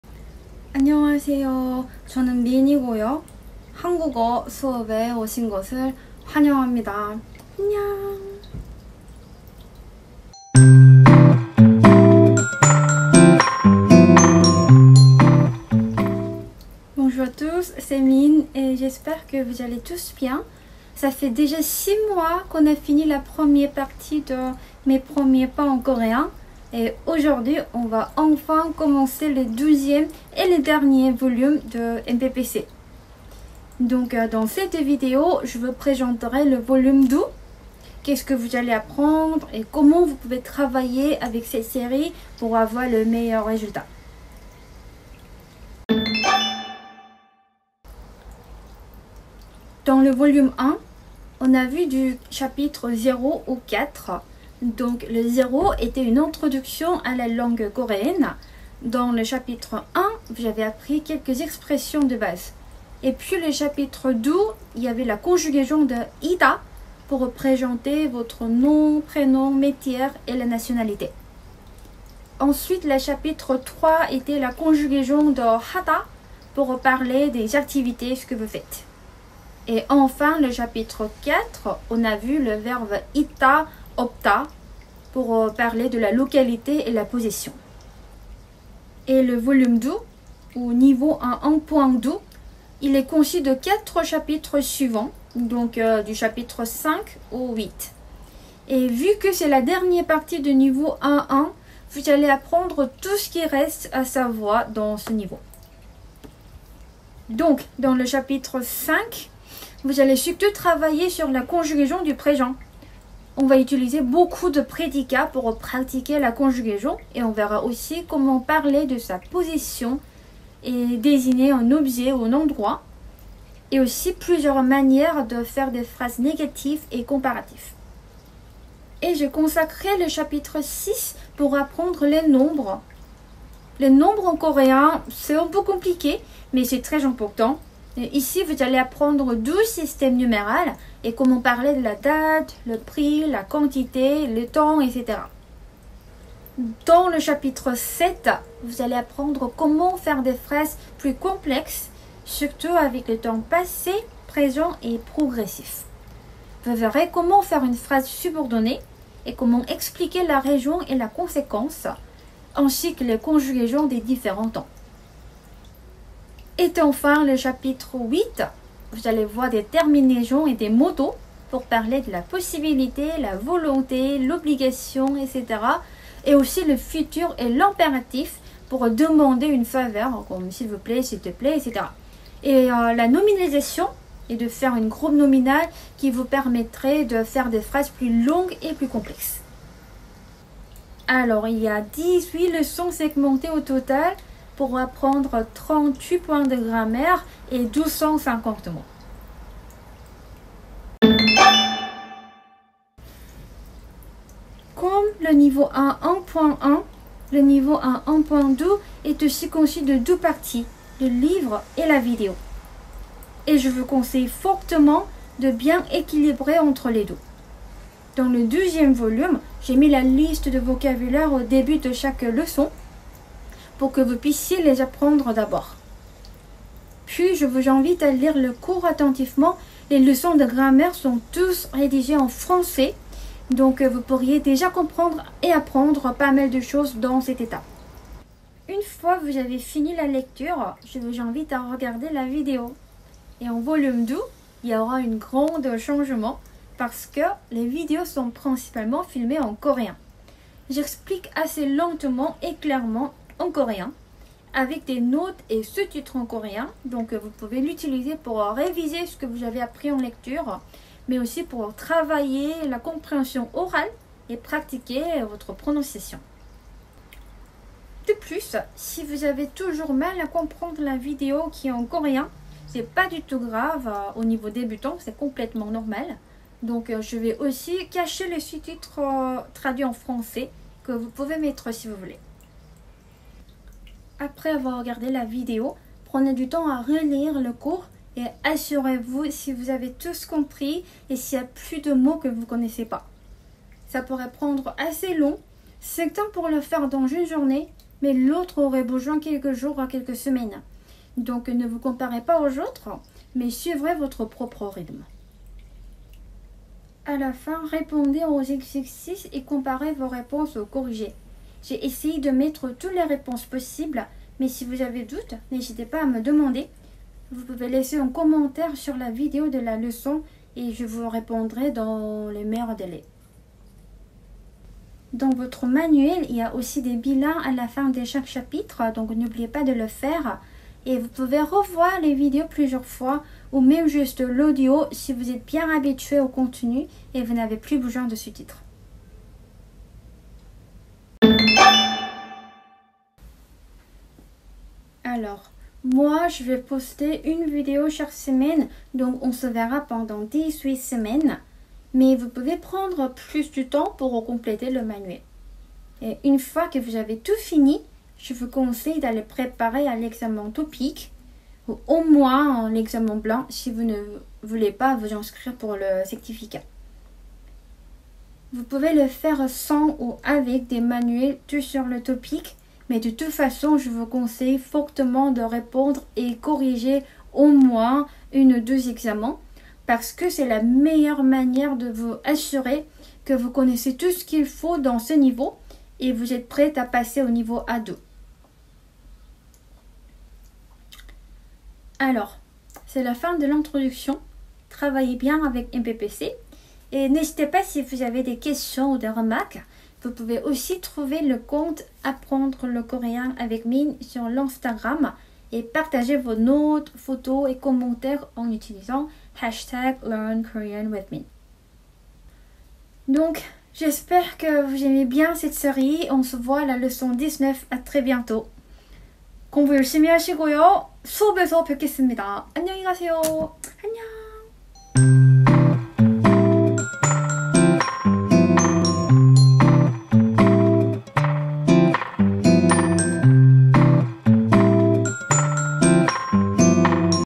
🇰🇷 [Mes Premier Pas en Coréen A1.2] MPPC A1.2를 시작합니다 !
여러분 안녕하세요 ~~ MPPC가 돌아왔습니다!
앞으로 18주간, 매주 토요일 LKWM 유투브 채널에서 만나요 :)

MPPC est de retour !
On se voit sur Youtube tous les samedis pendant 18 semaines :)